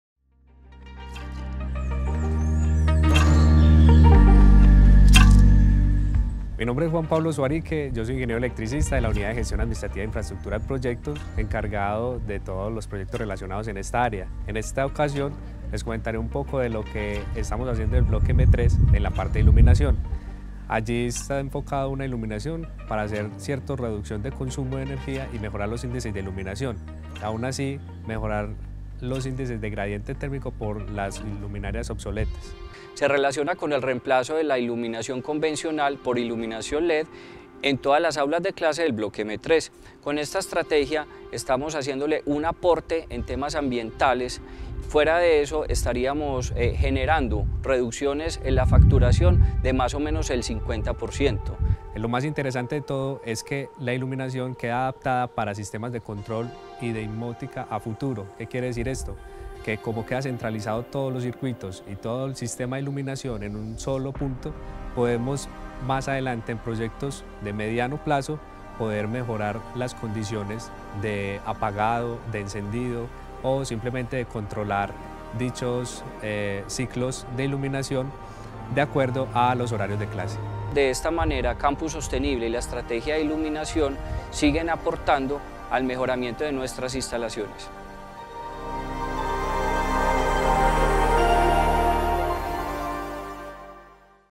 Proyecto de ingeniería en iluminación en el bloque M3
Este proyecto consiste en  la  actualización  tecnológica  de  toda  la  iluminación  del  bloque  M3,  incluyendo  aulas,  oficinas,  museo,  auditorios,  pasillos,  baños  y  laboratorios; además de la centralización de las redes eléctricas que suministran el  fluido  eléctrico  a  los  circuitos  de  iluminación.